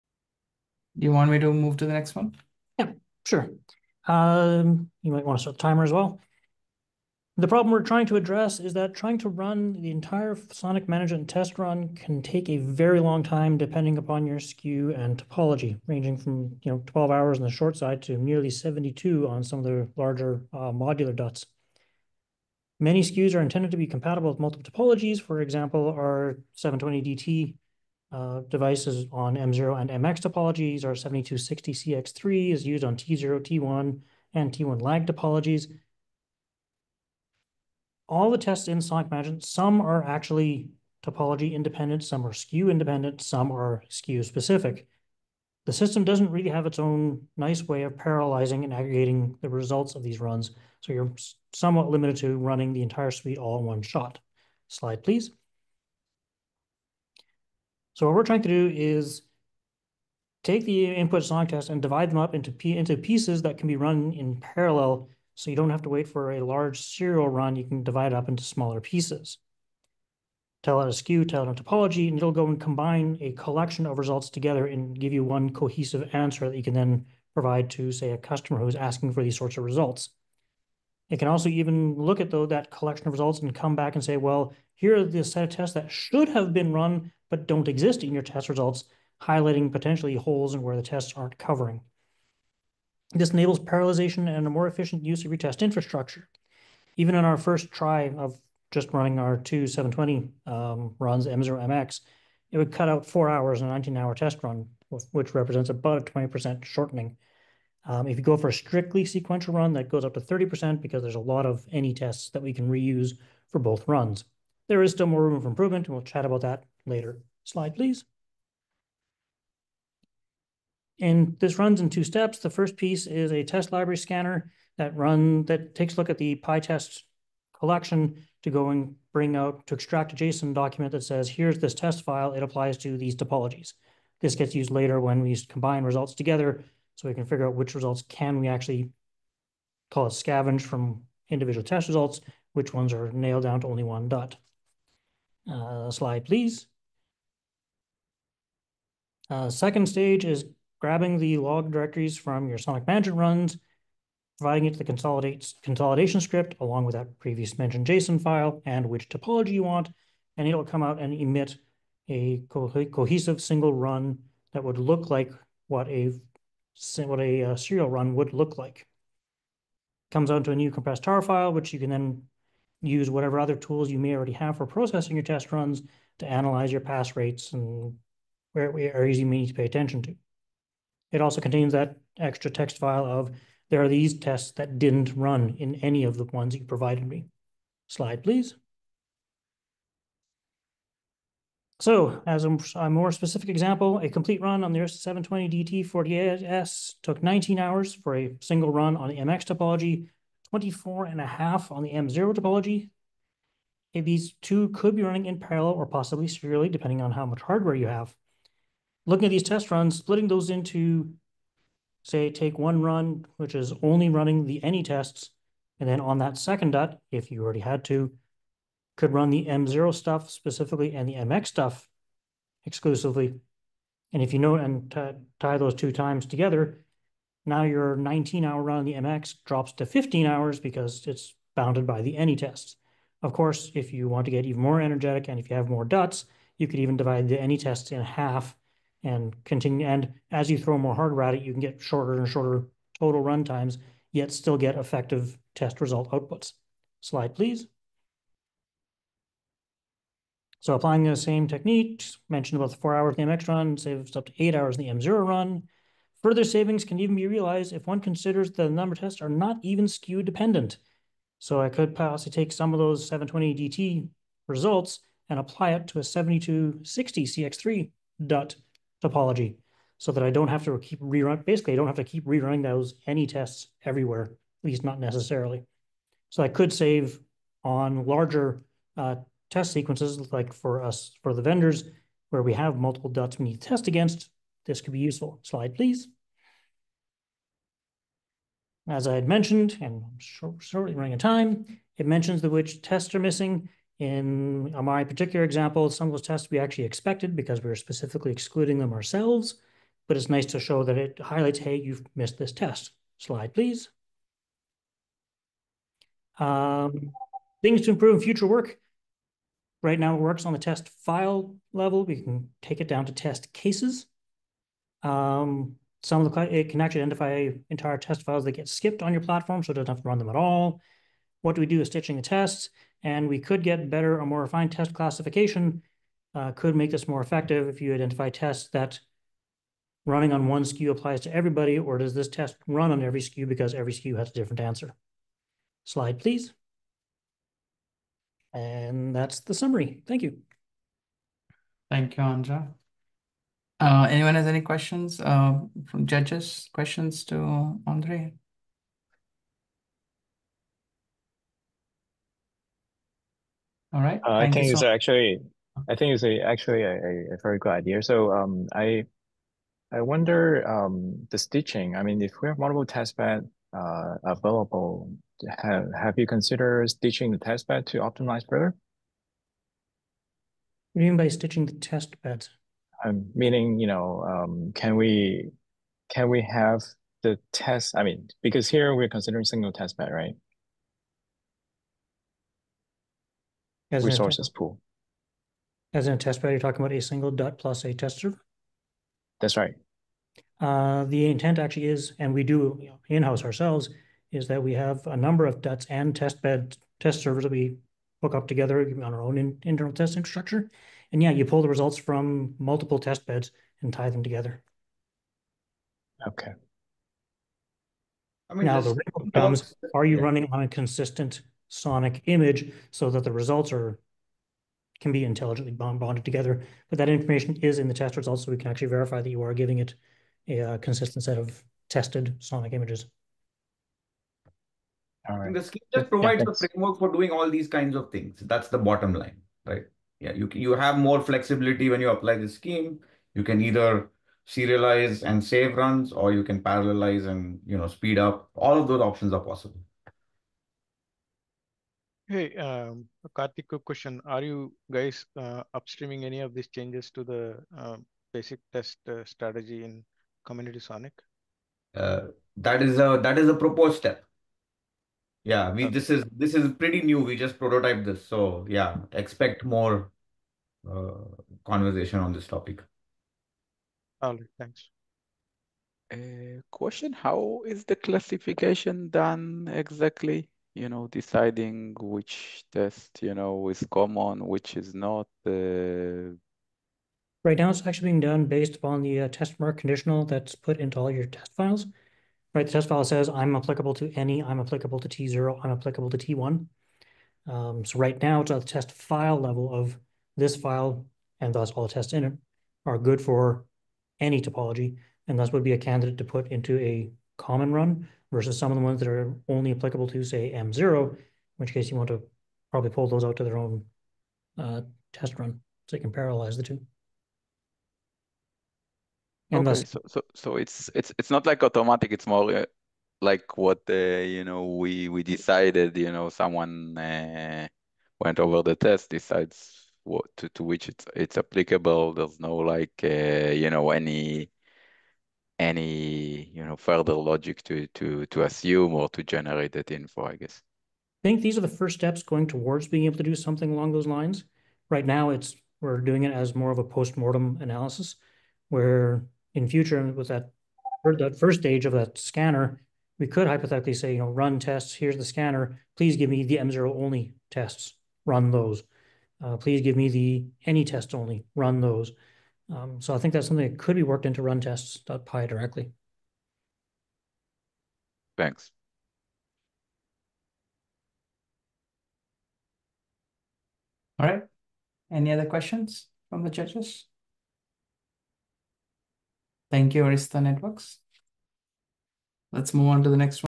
2024 SONiC Hackathon presentation: Team 8 presentation from Arista
Presentation Title: Parallelizing and reusing sonic-mgmt test
runs
Presenters: Kartik Chandran